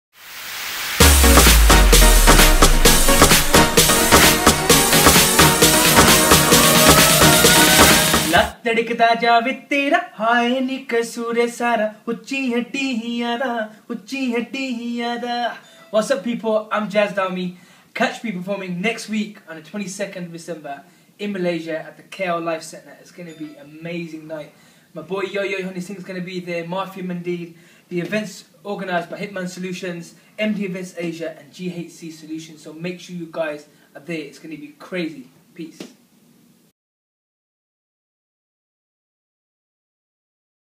YOYO2012 - Jaz Dhami Shoutout
YOYO Honey Singh & Friends ( Jaz Dhami | J-Star | Alfaaz | Money Aujla) live in concert (Malaysia) 2012.

Date : 22nd December 2012
Venue : KL Live Centre

2012's Biggest Concert Yet!!